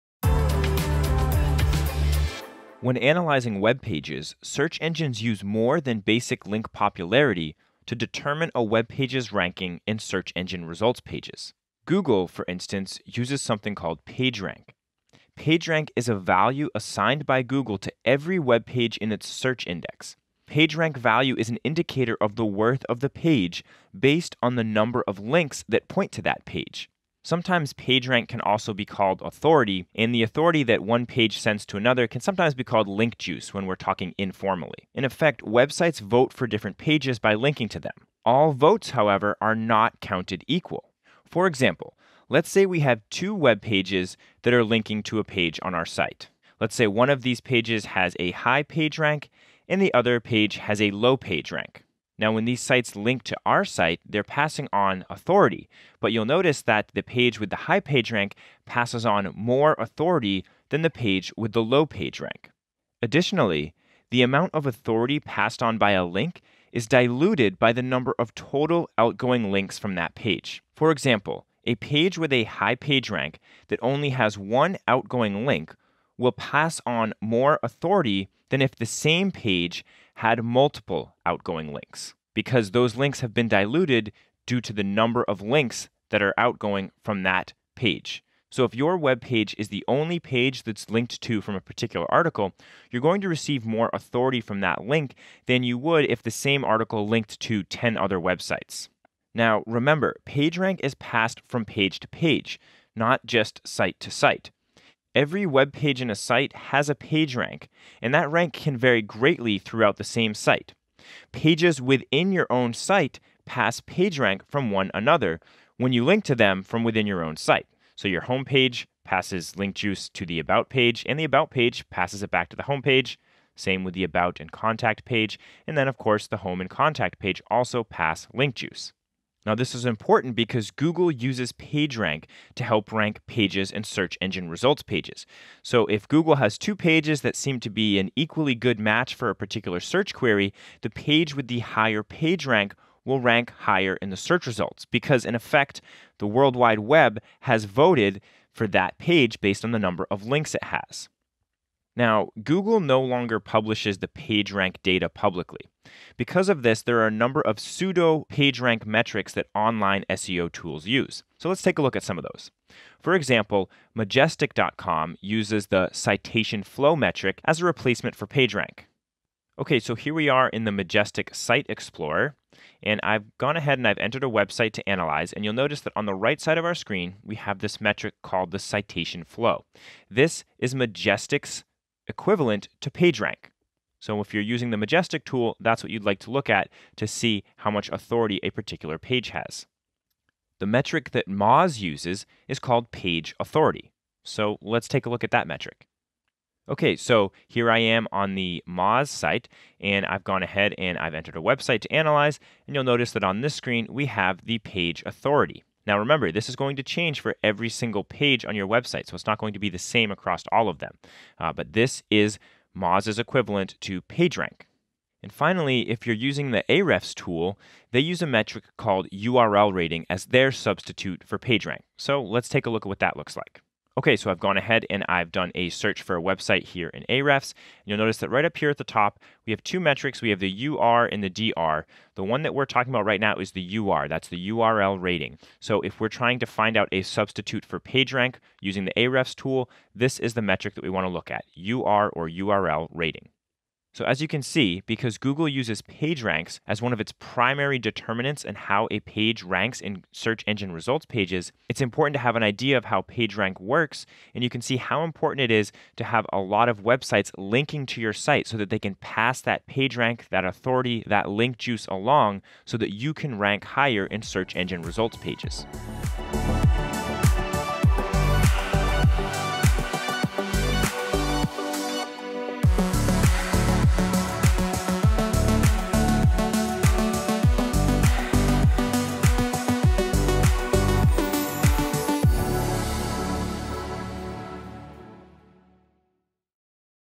SEO Tutorial - Understanding PageRank
Learn about the PageRank algorithm and how links pass authority to a web page. Explore more Search Engine Optimization courses and advance your skills on LinkedIn Learning:

This is an excerpt from "SEO: Link Building," a course on LinkedIn Learning taught by Anson Alexander. Anson is a blogger, author, SEO expert, teacher, and tech geek.

SEO: Link Building